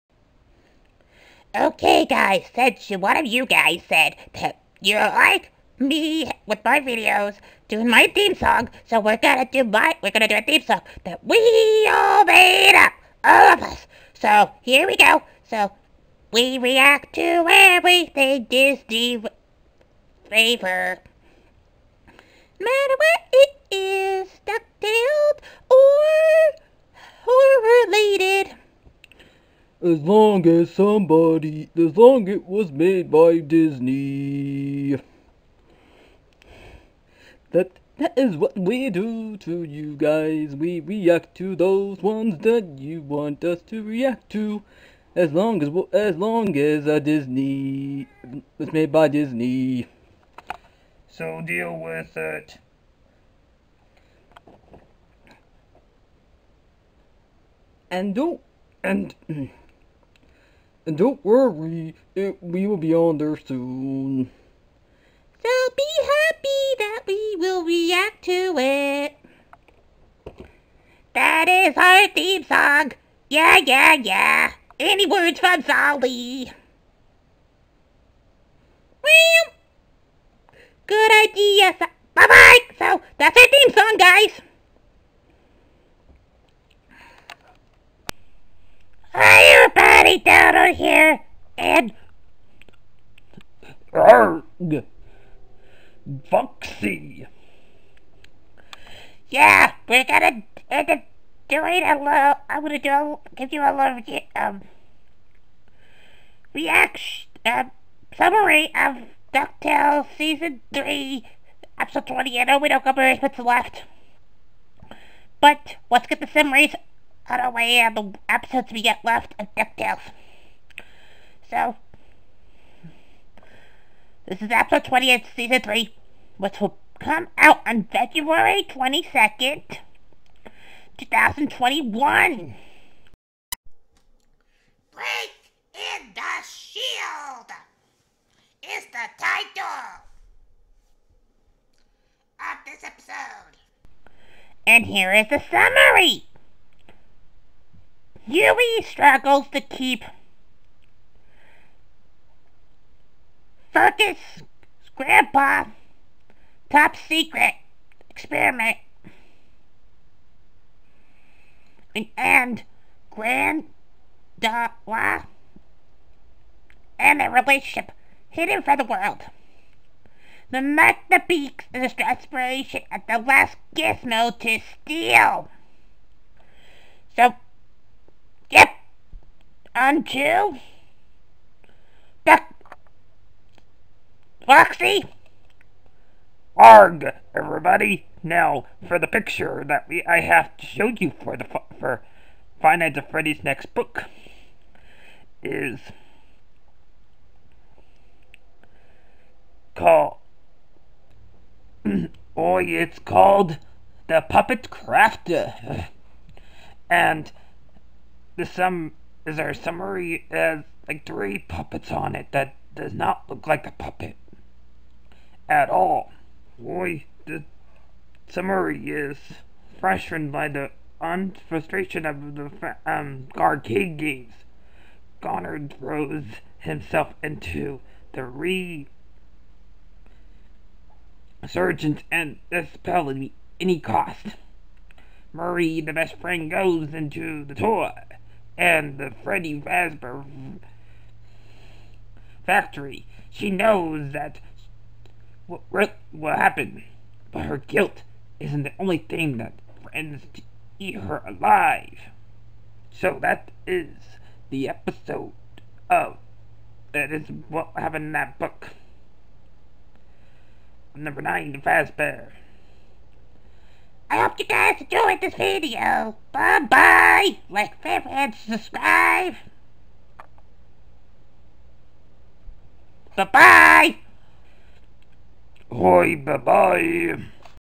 Donald and Foxy Give on Summary on Ducktale Episode and Frezbear Fight #9
Donald Duck give Ducktale Season 3 Episode 20 and Foxy Frezbear Fight #9
The Puppet Craver
Donald Duck